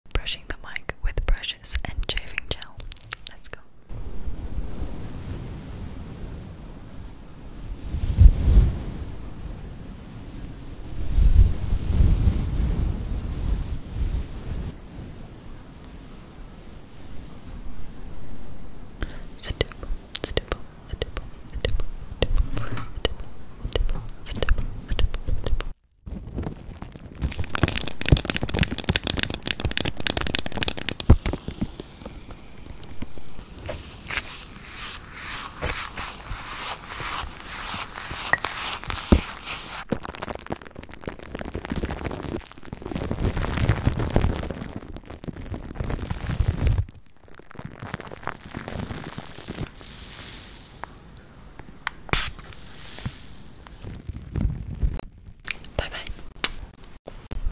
Brushing the mic with shaving gel!
Hi guys, welcome back! Today I'm making a very short video where I help you sleep. Hope you enjoy!